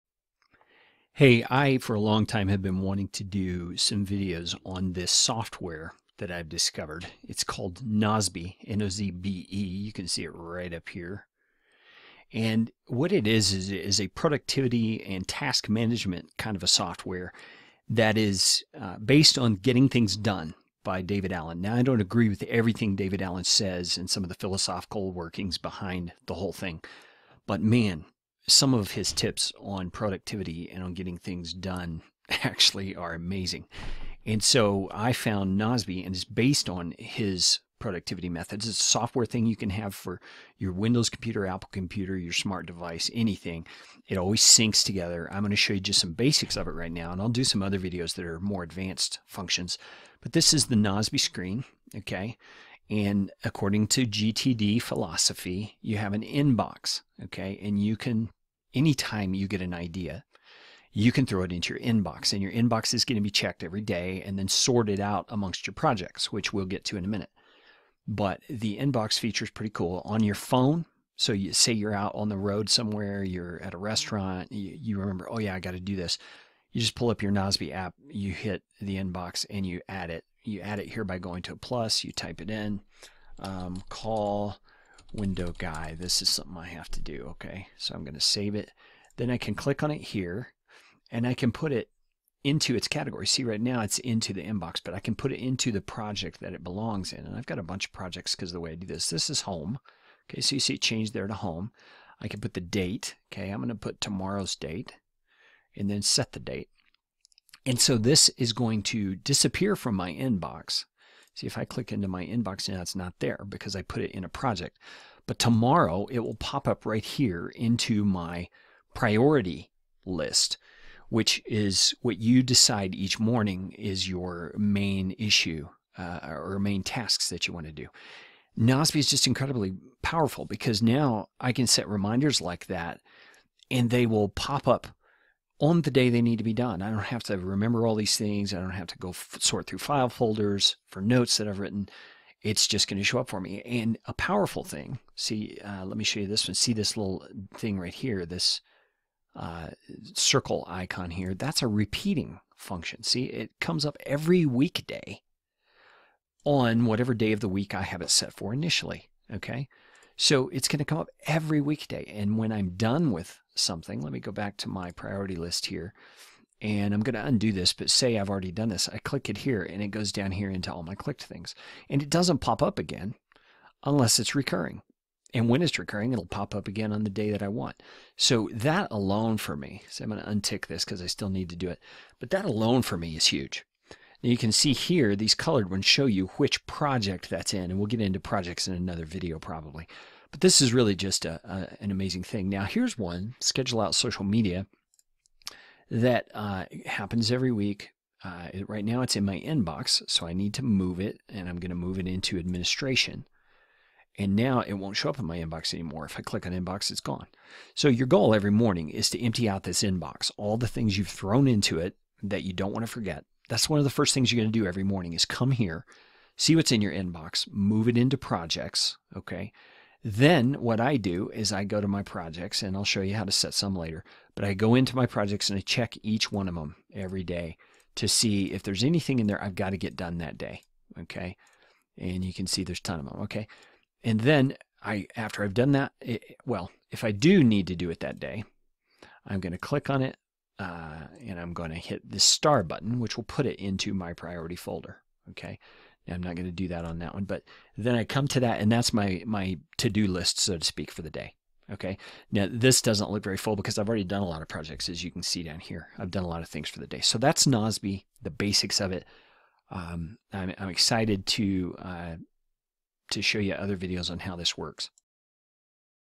Getting Things Done Software - Nozbe - 10% off - GTD, productivity and task management
- GET 10% OFF FOR LIFE (affiliate link)

Nozbe is based on the Getting Things Done methodology of task and life management - by David Allen. It's an amazing way of thinking and organizing your life to ensure that nothing falls through the cracks and everything gets done. This is the first of my videos giving a quick video of how Nozbe is laid out and how you can use it.

Use the link here (affiliate link) to get NOZBE for 10% over the lifetime of your account